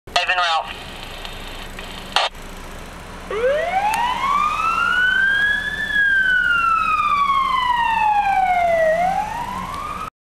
Engine 313 Responding
Here is 313 responding to an odor of something burning in a residence.  Turned out to be a small fire in the upstairs garbage can.  I got this on my way to school.